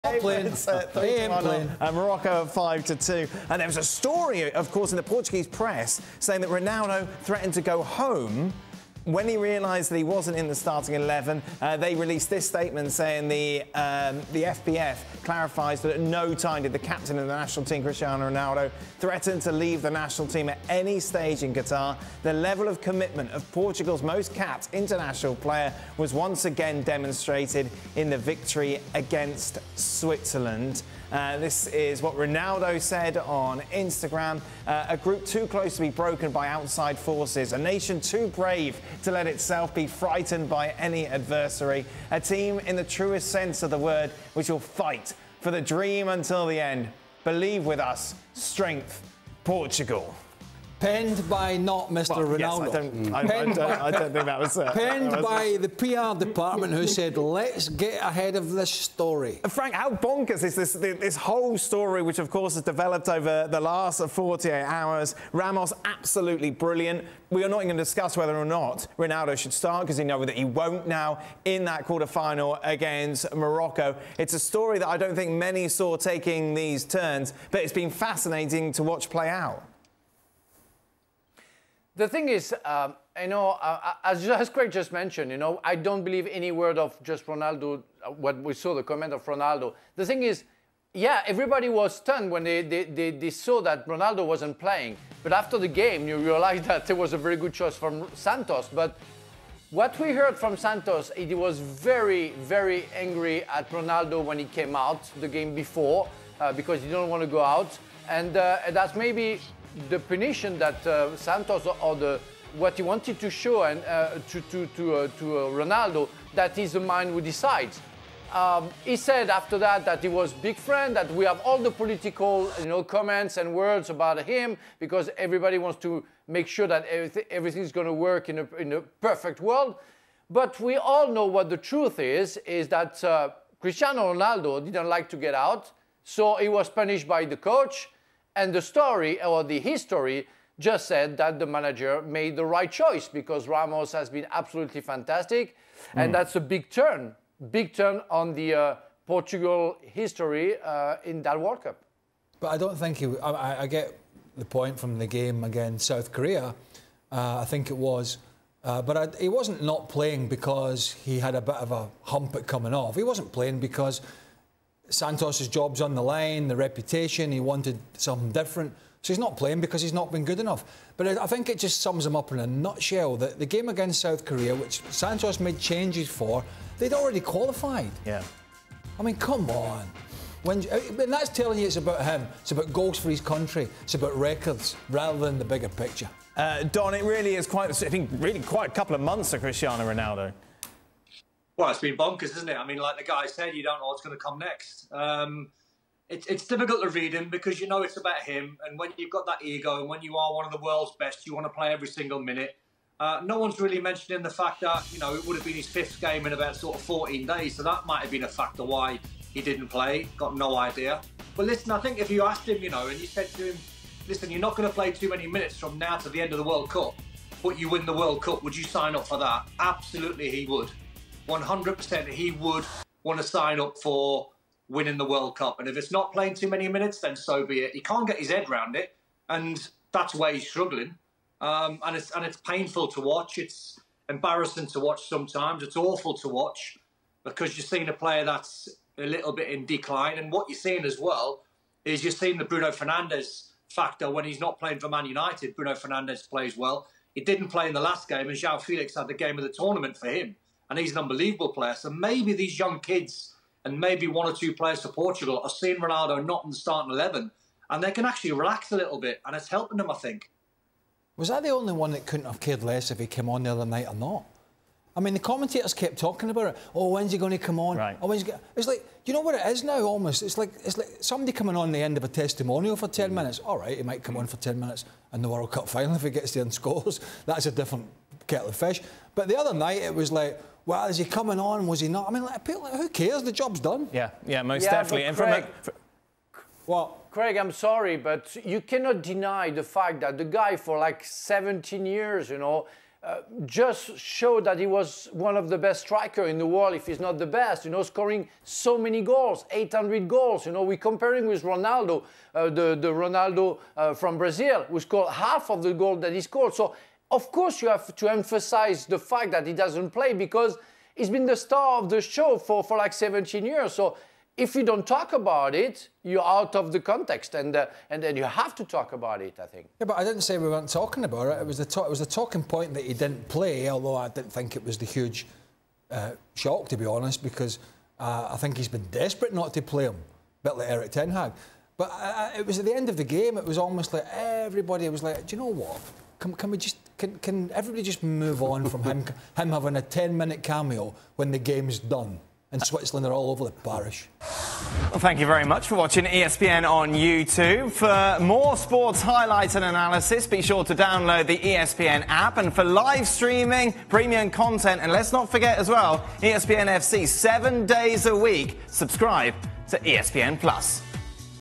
I don't believe a word from what Ronaldo said! - Frank Leboeuf | ESPN FC
The ESPN FC crew reacts to Cristiano Ronaldo's comments about a report that he threatened to walk out after not starting.